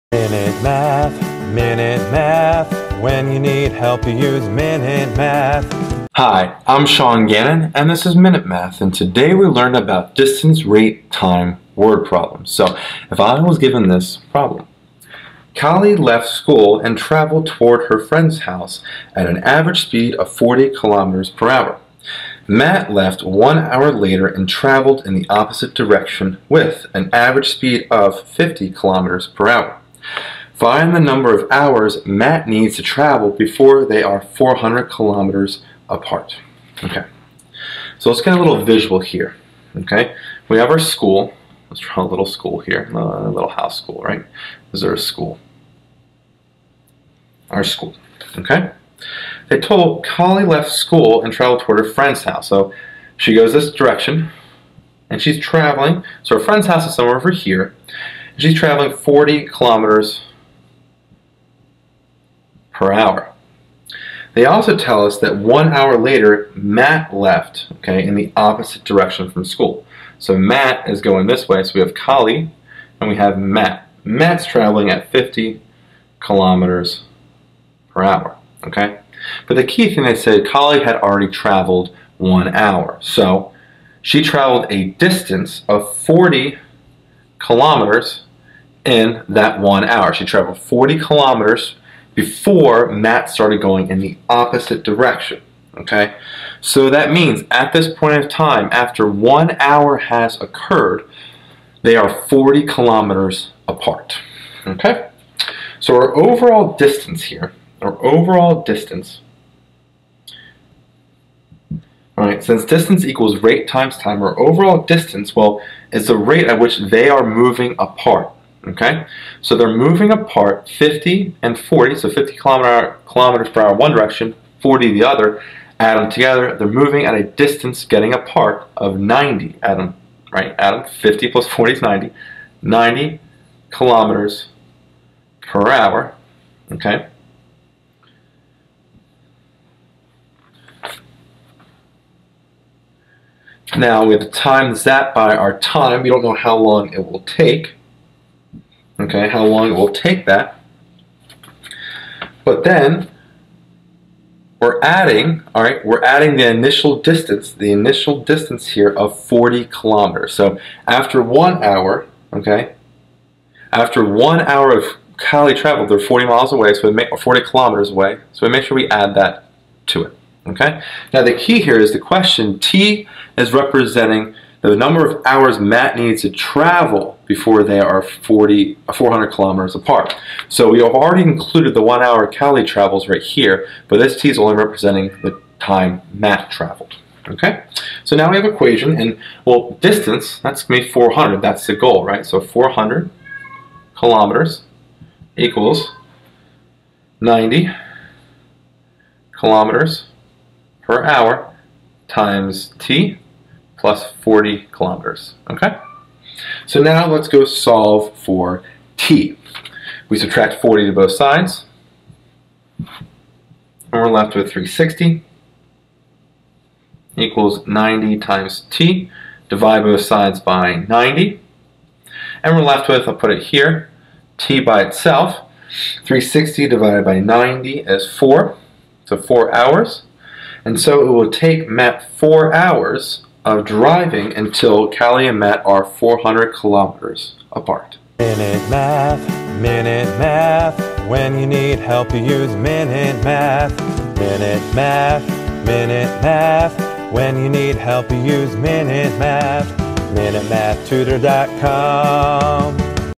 Kali left school and traveled toward her friend's house at an average speed of 40 km/h. Matt left...
In this math video lesson on Distance - Rate - Time Word Problems, I answer the following word problem: Kali left school and traveled toward her friend's house at an average speed of 40 km/h. Matt left one hour later and traveled in the opposite direction with an average speed of 50 km/h. Find the number of hours Matt needs to travel before they are 400 km apart.